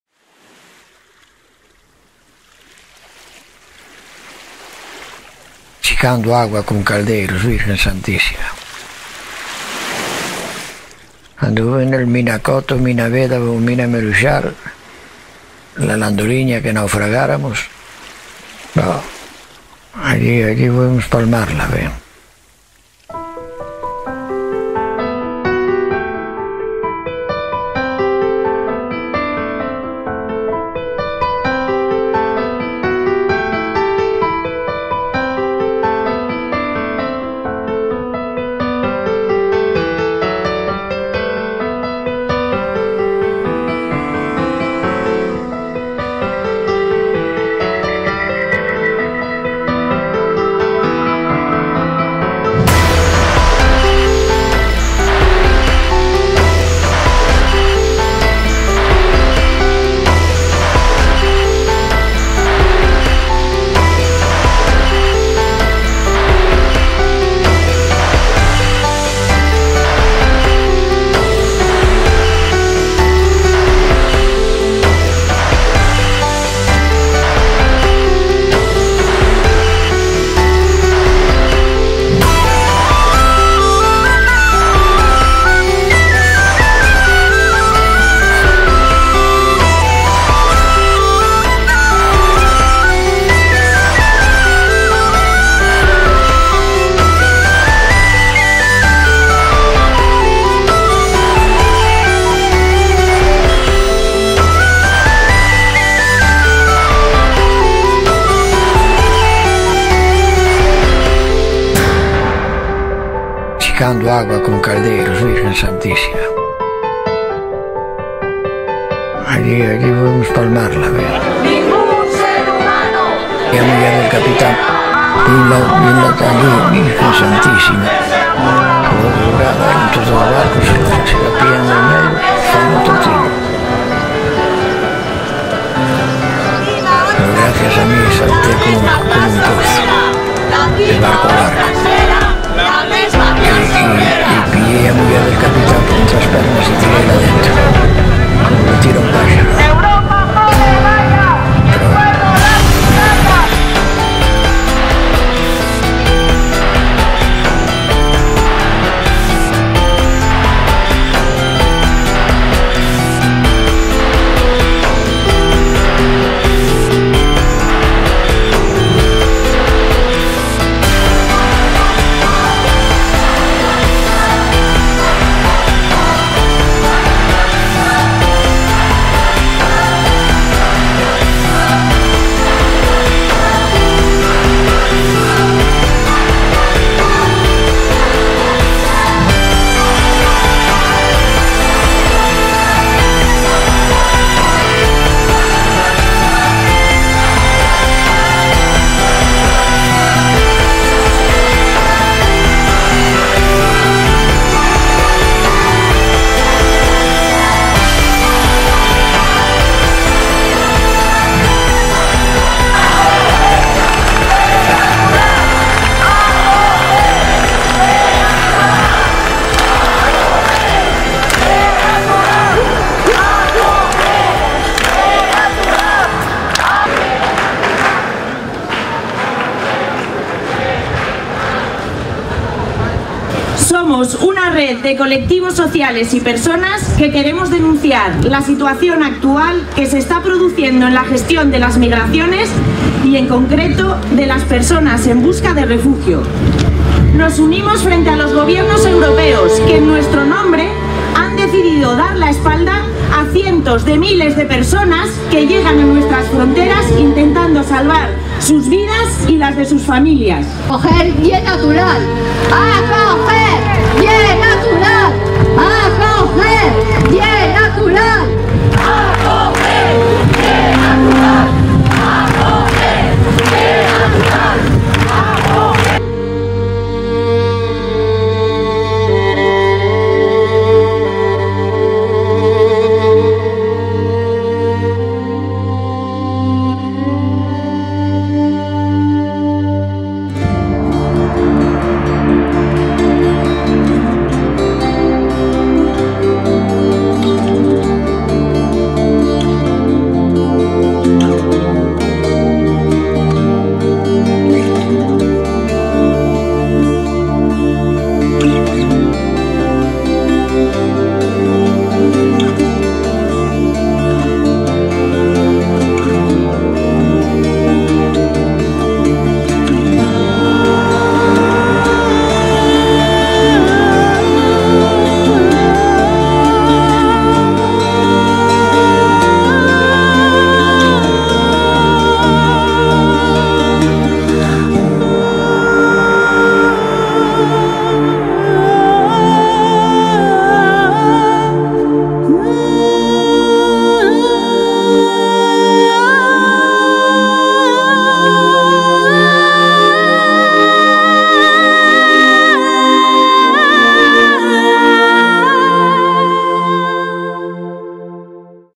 17_06_2017 MANIFESTACION ACOGER YE NATURAL (XIXON)
Manifiesto

Somos una red de colectivos sociales y personas que queremos denunciar la situación actual que se está produciendo en la gestión de las migraciones y, en concreto, de las personas en busca de refugio. Nos unimos frente a los gobiernos europeos que, en nuestro nombre, han decidido dar la espalda a cientos de miles de personas que llegan a nuestras fronteras intentando salvar sus vidas y las de sus familias.

Nos unimos para defender los derechos humanos, aquellos que todas las personas tenemos por el hecho de nacer y que ahora son sistemáticamente vulnerados por gobiernos que deberían protegerlos. Esos gobiernos que se dicen democráticos fomentan guerras, construyen muros y desarrollan políticas que provocan que miles de personas sufran situaciones indignas en campos de refugiados.

Ellos han convertido el Mediterráneo en la fosa común más grande del mundo. Desde el año 2000 más de 30.000 personas han muerto en su intento de llegar a Europa.

Nos juntamos para decir Basta ya. Basta ya de pedir papeles al que quiere salvar la vida, Basta ya de encerrar a quien hay que acoger, Basta ya de dejar morir ahogados a quienes huyen de una guerra, Basta ya de lavarse las manos por los niños y las niñas desaparecidos/as, por las mujeres violadas y por todos los muertos inocentes.

Denunciamos el pacto de la vergüenza entre Turquía y la Unión Europea que, hace ya un año, impuso el cierre de nuestras fronteras a personas que huyen de la guerra.

Denunciamos el incumplimiento del Gobierno español de los acuerdos para acoger a las personas refugiadas, la violencia ejercida en las fronteras de Ceuta y Melilla, la expulsión inmediata de personas sin aplicarles la protección que ofrece la ley y el internamiento de las personas inmigrantes en los CIEs (Centros de Internamiento de Extranjeros).

Recordamos que en nuestro país, hace 80 años, también fuimos personas refugiadas.

Recordamos que antes nuestr@s abuel@s, y ahora nuestr@s hij@s, también fueron y son personas emigradas.

Recordamos que tener nacionalidad española, no significó nada para proteger los derechos del pueblo saharaui.

Recordamos que no es un delito huir de una guerra.

Que no se llame delito a un derecho, que no se busquen excusas para negar la humanidad, que no se violen más los Derechos Humanos por razones económicas y xenófobas.

No podemos olvidar que el asilo es un derecho. Por eso nos solidarizamos y damos la bienvenida a todas las personas que se ven forzadas a abandonar su país para no perder la vida a causa de guerras, catástrofes naturales o persecuciones por razones de etnia, orientación sexual, política, religiosa, nacionalidad o género.

Por todo esto exigimos a la Unión Europea y al Gobierno español que deje de avergonzarnos y estén a la altura de la ciudadanía. Que pongan fin a esta situación de injusticia y dejen de vulnerar los derechos humanos amparándose en leyes que no protegen al débil y no representan la solidaridad del pueblo europeo.

Exigimos a la Comunidad Autónoma y los Ayuntamientos de Asturies que presionen al Gobierno español para que se respeten los Derechos Humanos, en especial el Derecho de Asilo y el Derecho a la Libre Circulación de personas, que garanticen la inclusión y el desarrollo social de las personas migrantes, en nuestra tierra, y que apliquen medidas contra el racismo y la xenofobia.

Porque Acoger Ye Natural.